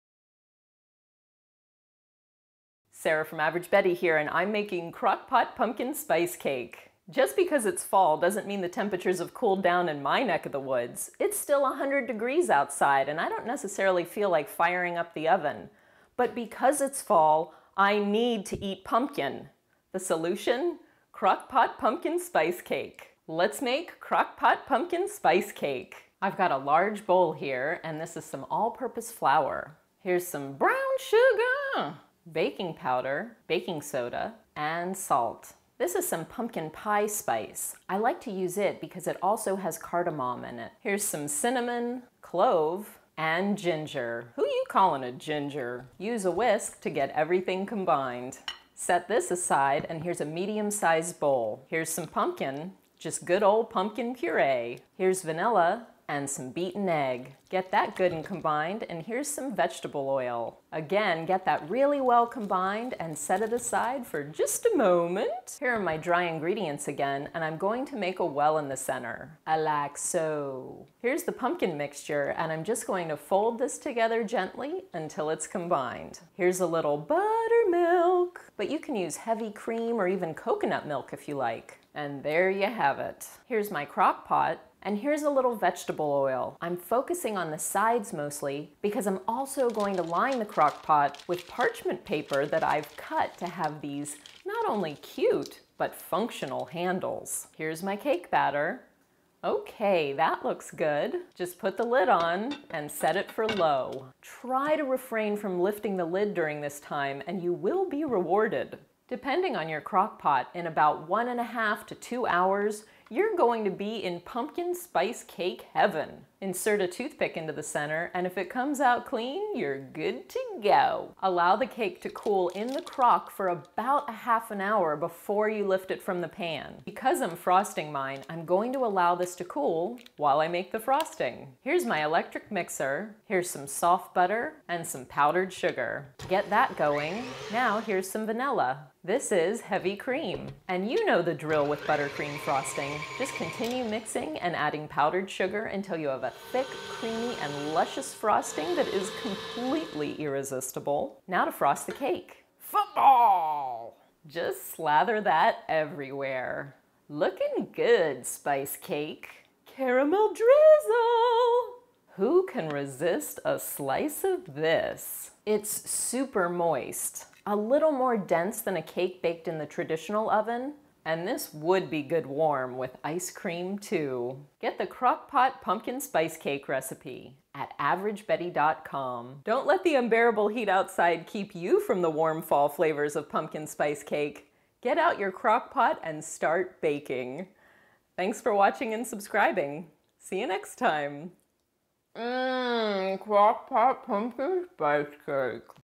Crock Pot Pumpkin Spice Cake - How to Make Cake in a Slow Cooker
Because it's still a hundred degrees outside, Sara experiments with making Pumpkin Spice Cake in the Crock Pot.